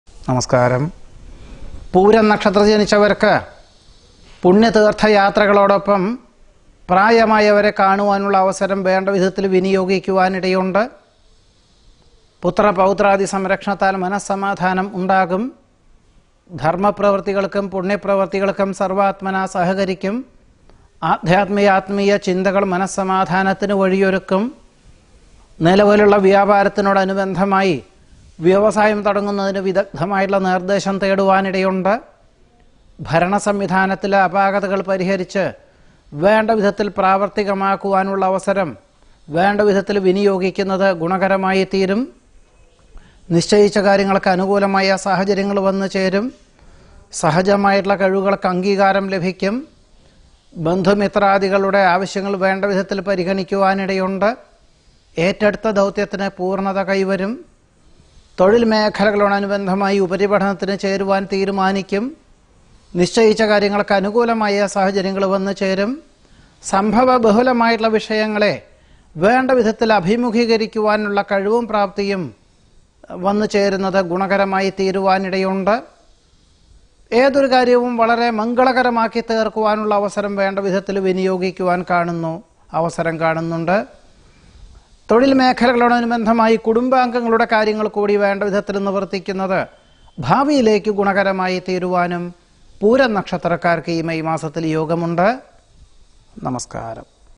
Pooram I May 2016 I Kanippayyur Narayanan Namboodiripad
Nakshatraphalam in Malayalam - Monthly Prediction by Sri Kanippayyur Narayanan Namboodiripad